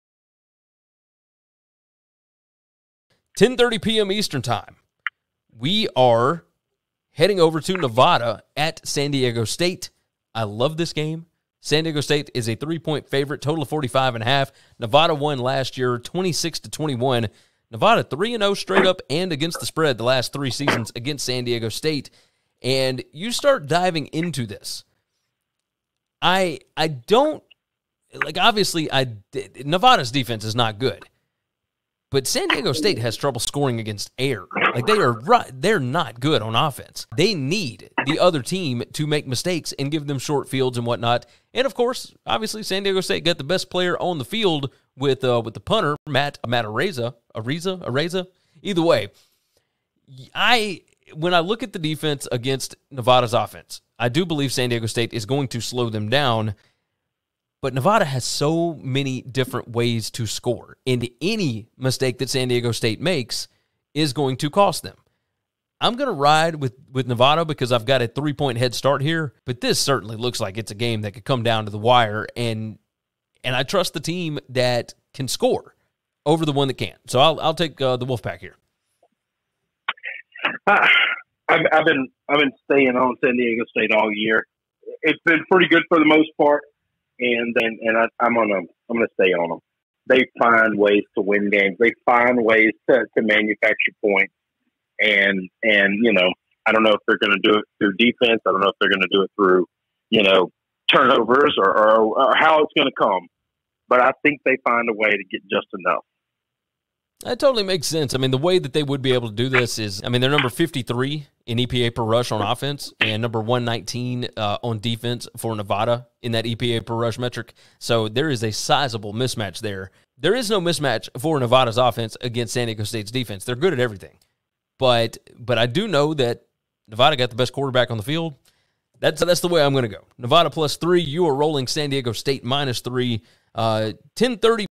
Nevada vs San Diego State Picks Against the Spread Predictions 2021 College Football
Gary & Chris give a quick preview, predictions & their picks against the spread (ATS) for the Nevada vs San Diego State college football matchup this weekend, Week 11!